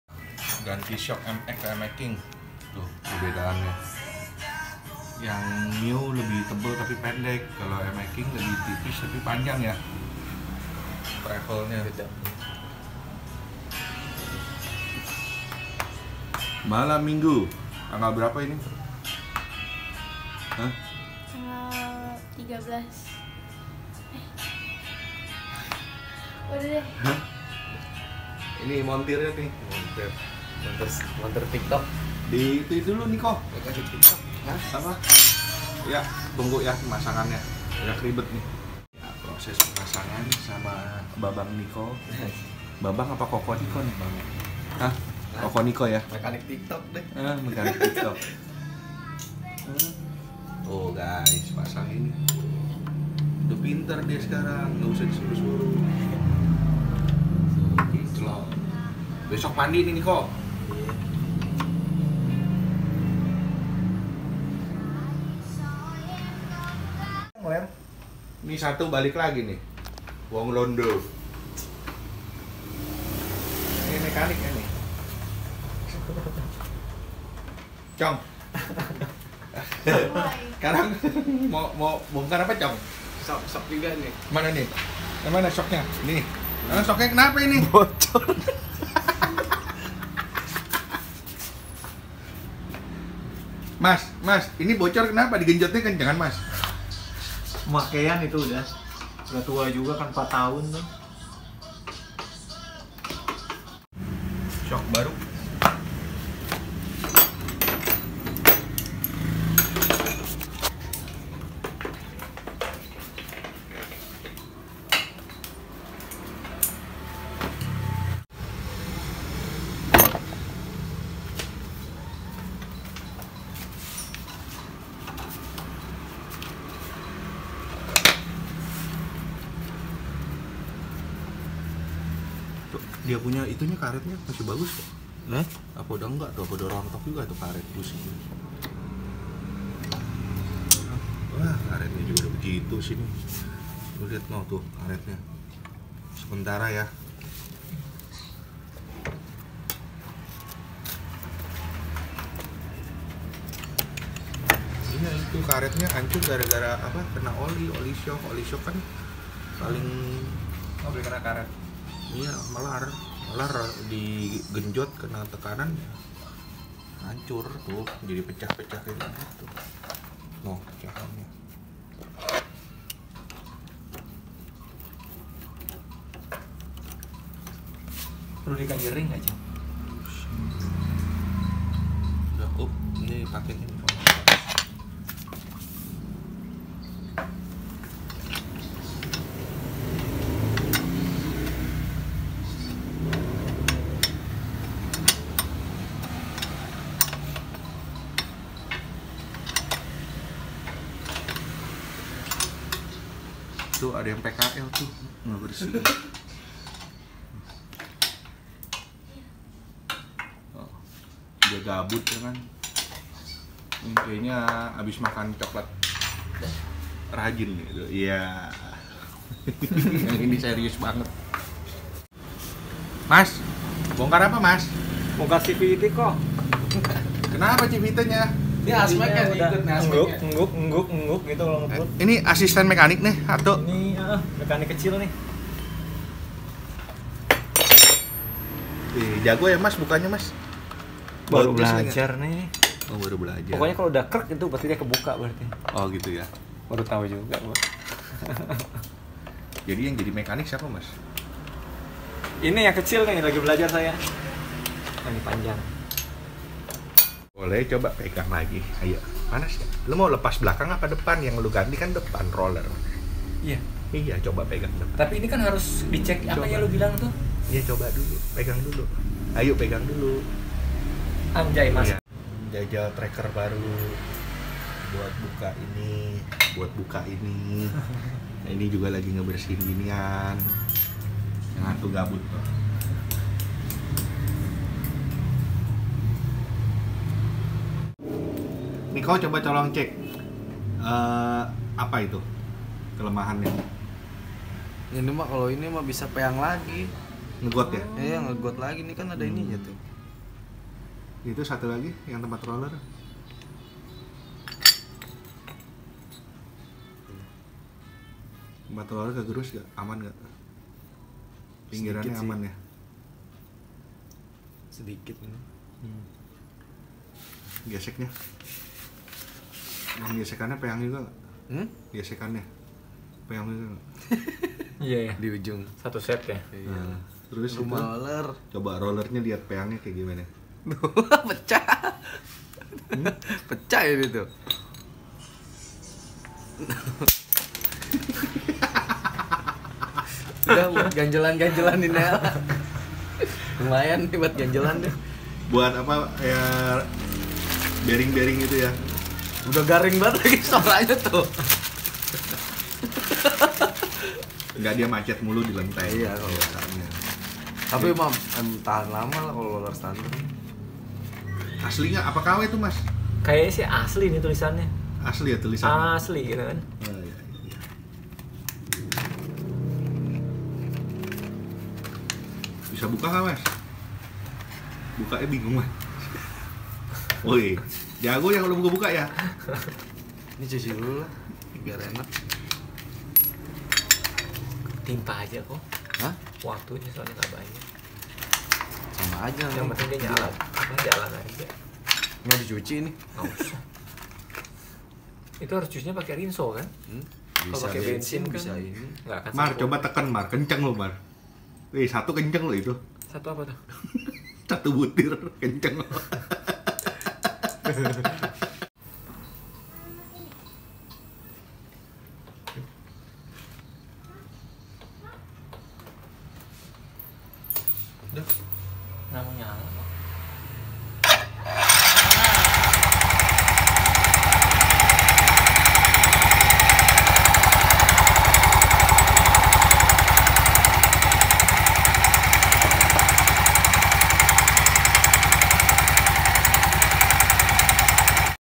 Bongkar pasang dimalming 13/8-2022
Malming gabut jd family time bareng anak dan teman yg bawa anak jg. Sambil bongkar pasang shock breaker, bersihin master rem Jupiter mx new sekalian pasang kampas rem..

Sm motor kawan soulgt125 pasang shockbreaker jg dan ganti roller yg dah peyang2..